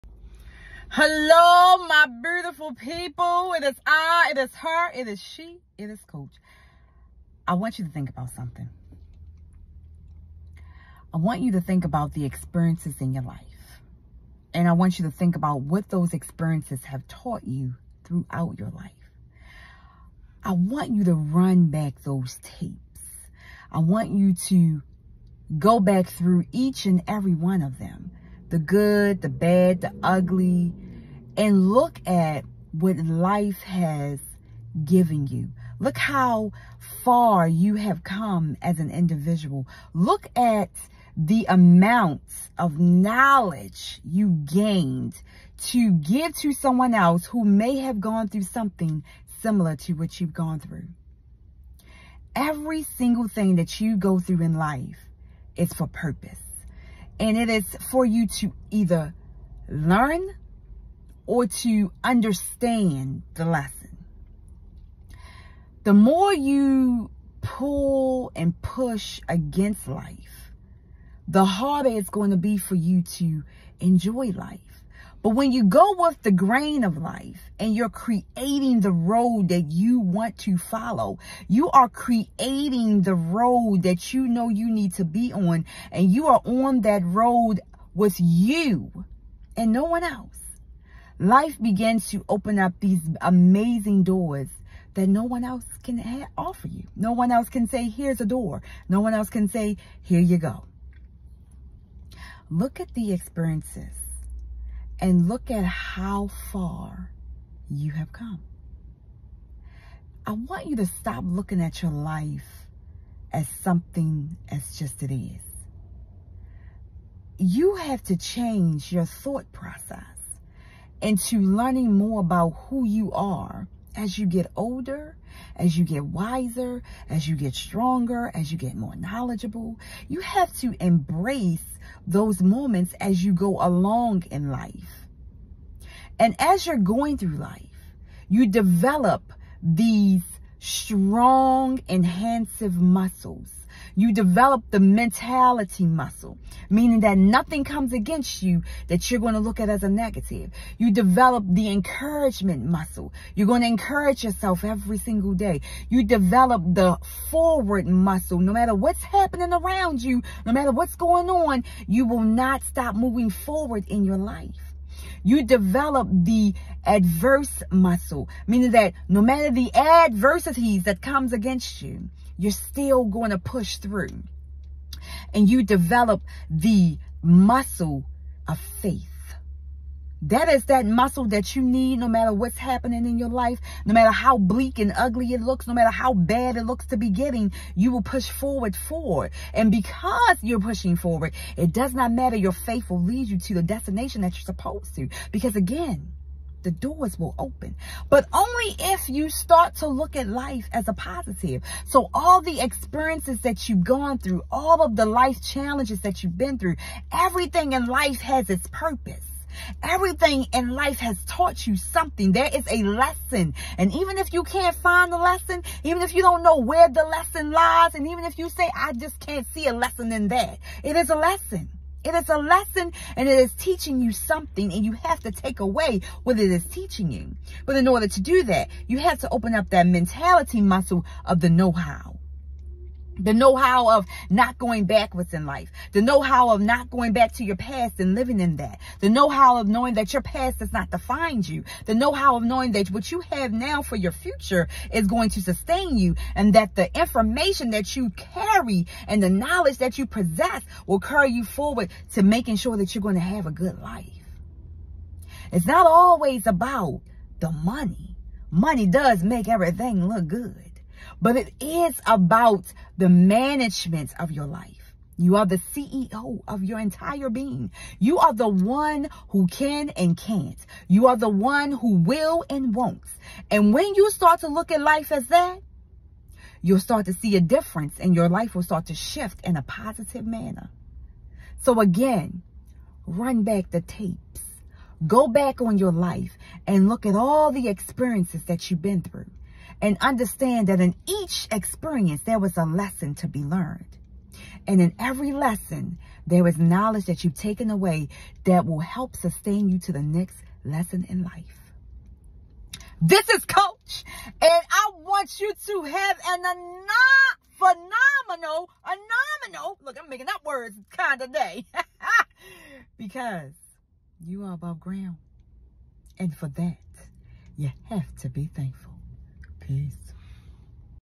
Run Them Tapes Back! Learn the Lesson & Thrive in Your Life!
You can’t control things that are out of your control! It’s just not possible, but you can learn great lessons from those uncomfortable moments.

Everything in your life has been and continues to be an experience that you will and can learn from. In this video I’m going to Run back those tapes!

The lessons learned can & does teach you to acknowledge and appreciate where you have been to understand where you are headed!